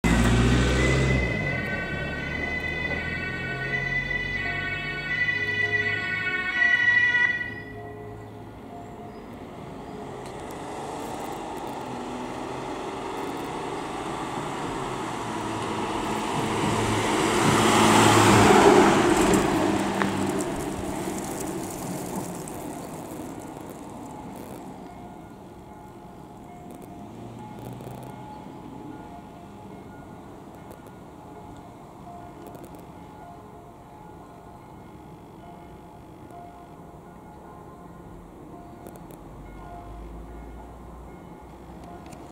RTW DRK Rettungsdienst Neuss - Rettungswache Süd
Hier sehen sie einen RTW des DRK Neuss auf Einsatzfahrt in Neuss Norf .

Aufnahme Ort : Neuss-Norf
Uhrzeit : -
Datum : -

Zu sehende Fahrzeuge :

- 9-RTW-1

Kunsturhebergesetz(KunstUrHG)Beiwerk.